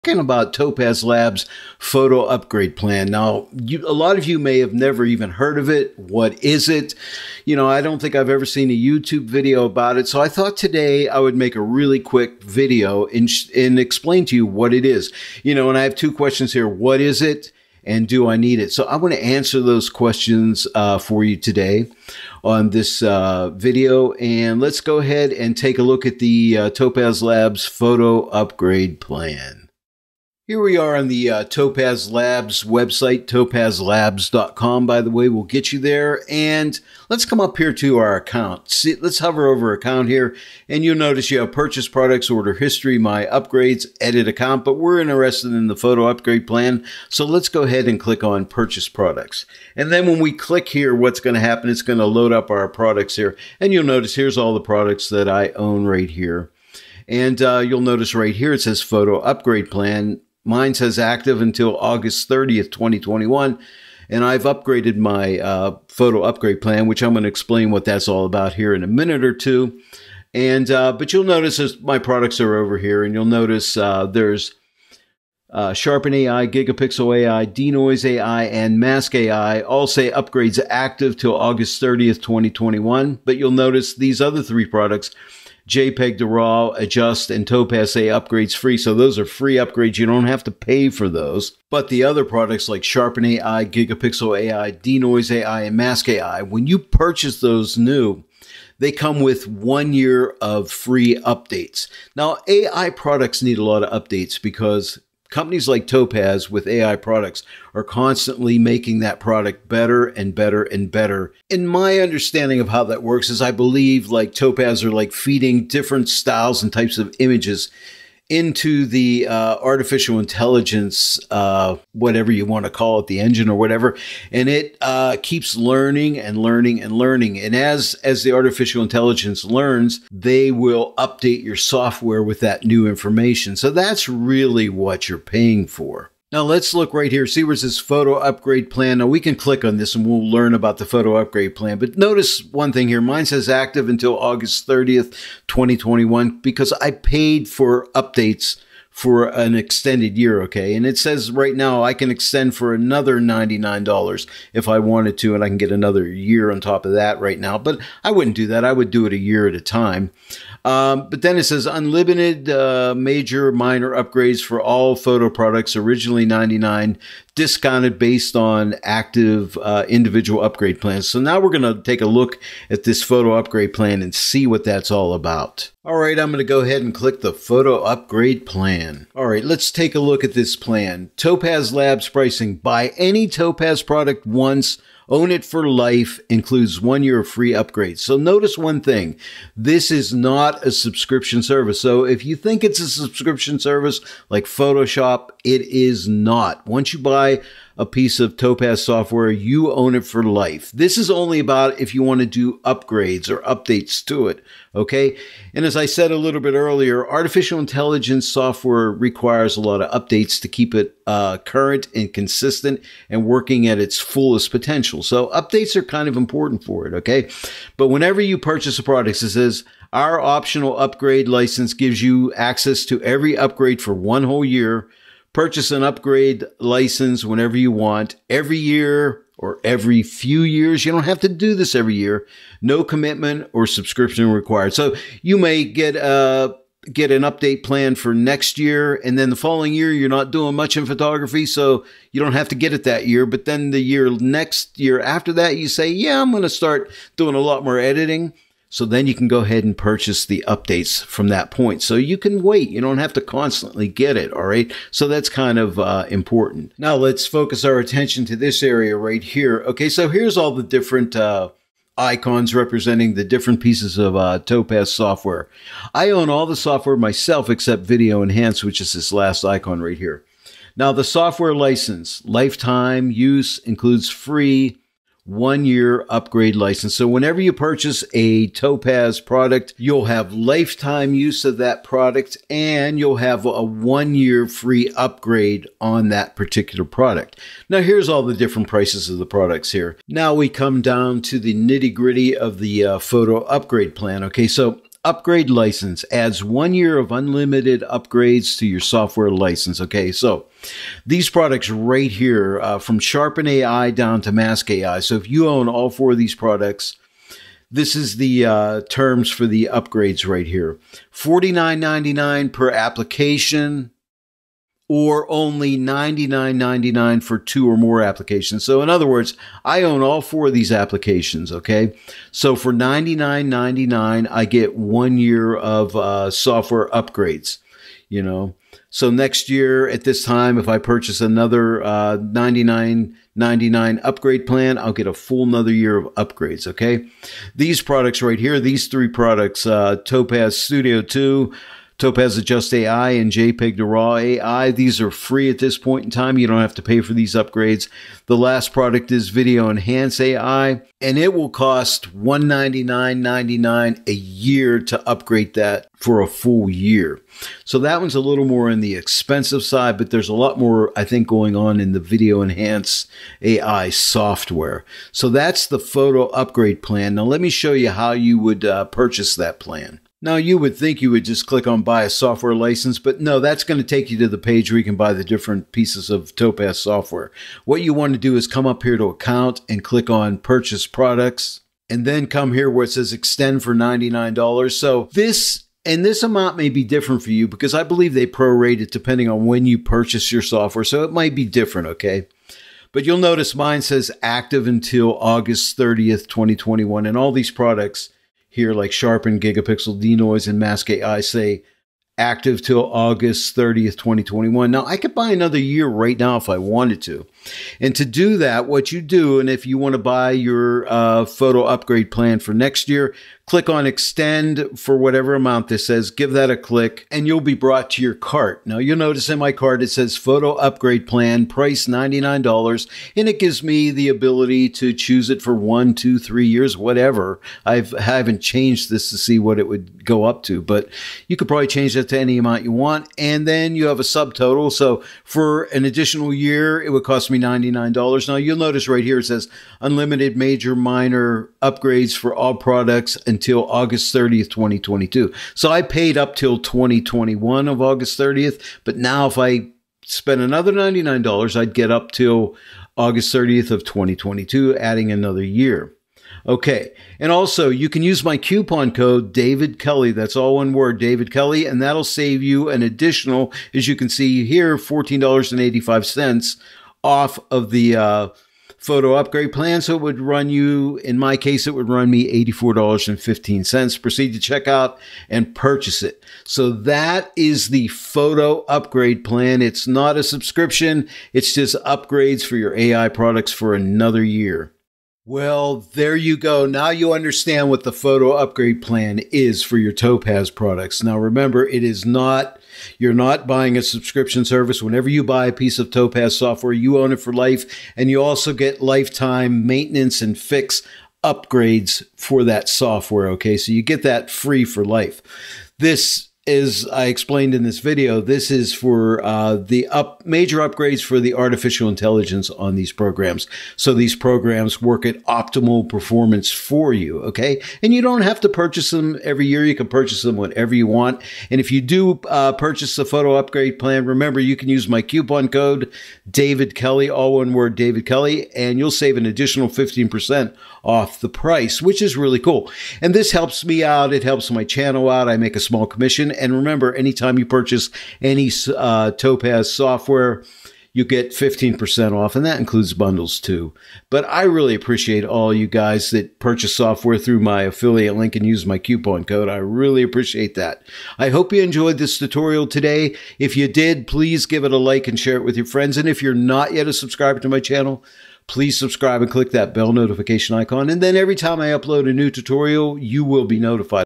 TOPAZ LABS PHOTO UPGRADE PLAN: What is it? Do I need it?
What is the TOPAZ LABS (PHOTO UPGRADE PLAN) and do I need it? Find out on on today's video...